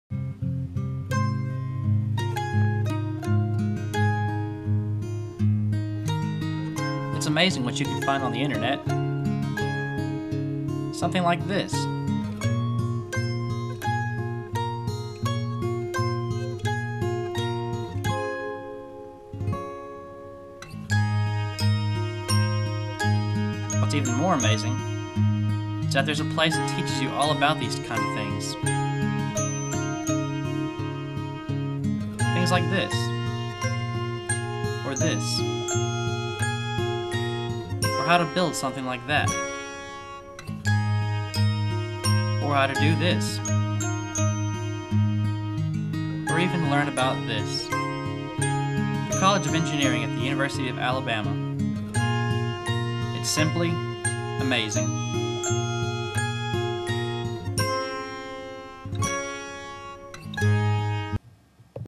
Amazing - iphone spoof
awmitchell1

A spoof of the iphone commercials.
Intended audience: prospective UA engineering students.